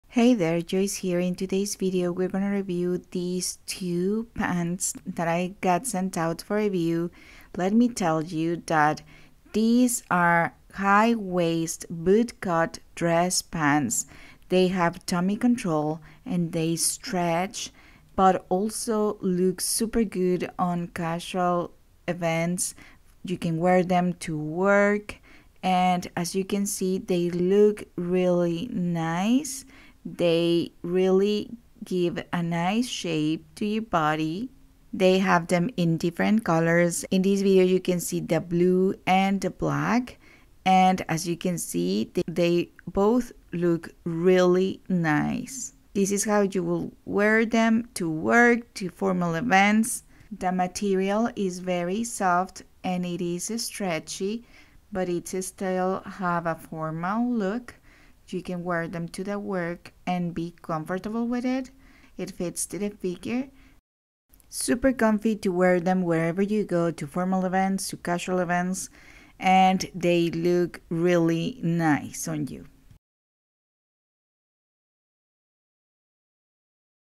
Casual Work Pant for Women’s High Waist Bootcut Dress Pants with Tummy Control
Aurefin Women’s High Waist Bootcut Dress Pants, Tummy Control, 4-Way Stretch Pull on Casual Work Pants for Women.